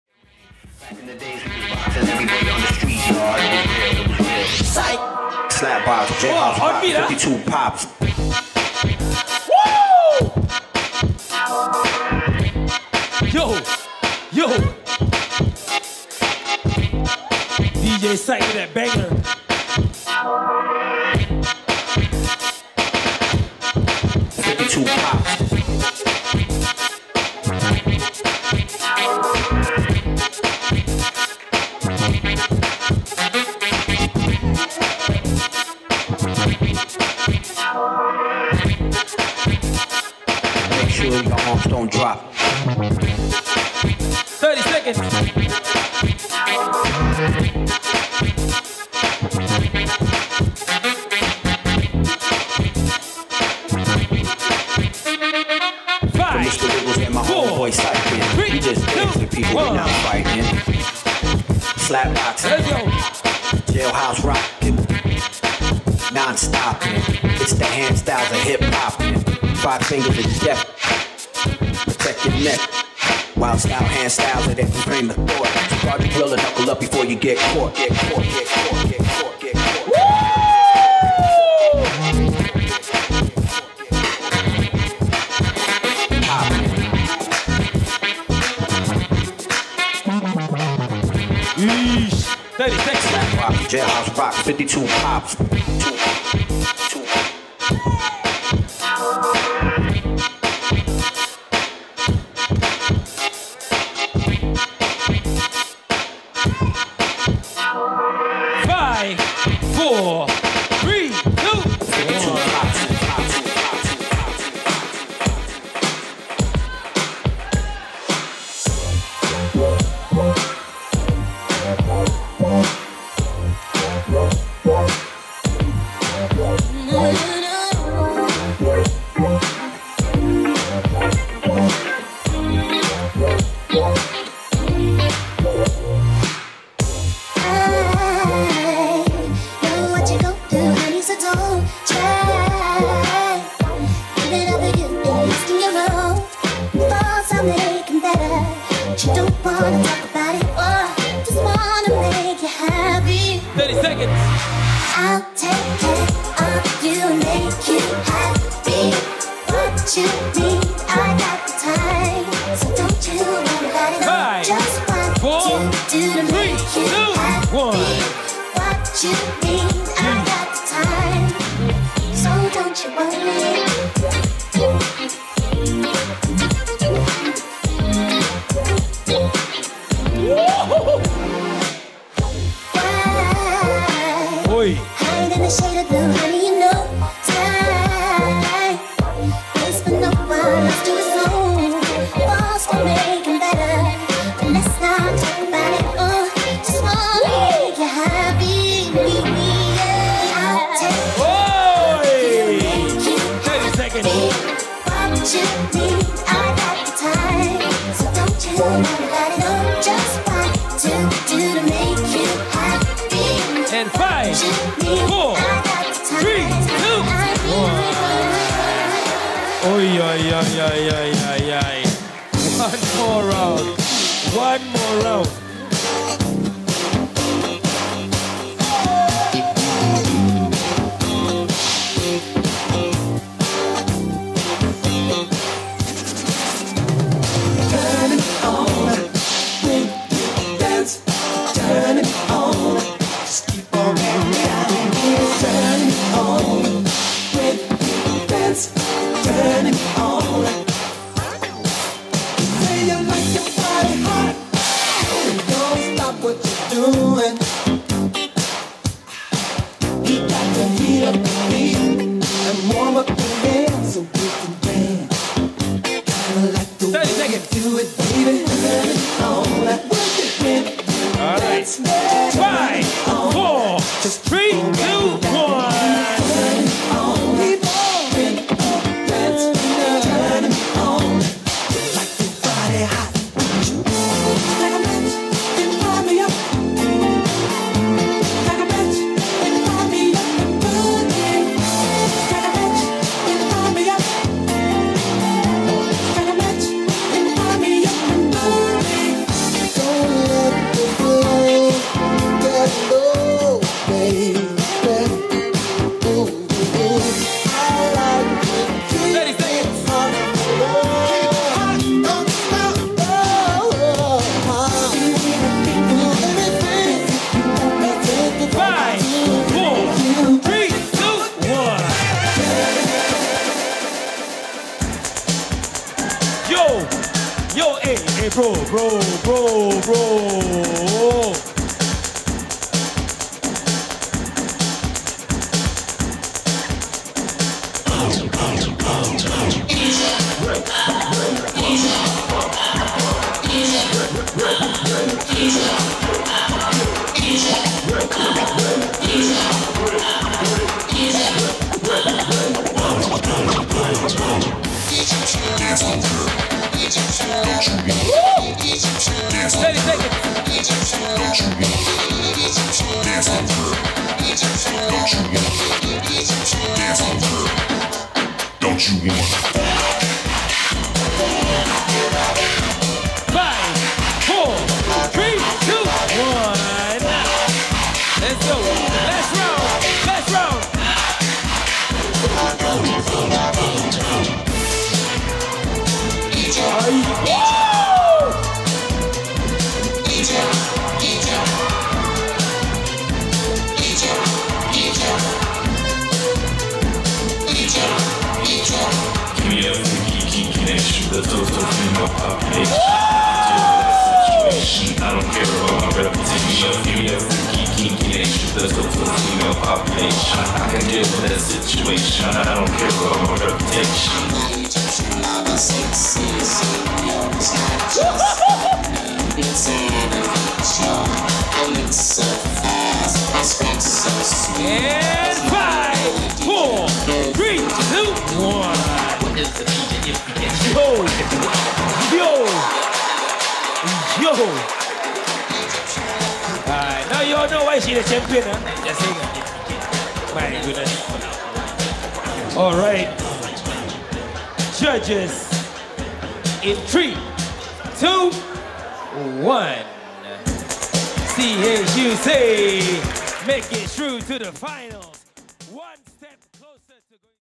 C_MILLE vs CHUC | Popping Top 4 | WDC Singapore 2024 | RPProds
Organized by  @DanzPeople

Singapore takes centre stage for the first time as it hosts the first edition of World Dance Colosseum (WDC) Singapore. WDC Singapore features intense 2v2 showdowns in Hip Hop, House, Popping, Waacking, and 1v1 Under 16 Battle. Winners from this electrifying event will be seeded for the prestigious WDC 2024 FINAL in Japan.

Judges:
D Soraki (JPN)
Hiro (JPN)
Marzipan (SG)
Racheezels (SG)
Aaron Tan (SG)

Filmed by TK
Edited by Richard Prayoga
Thumbnail Photo by Jezzie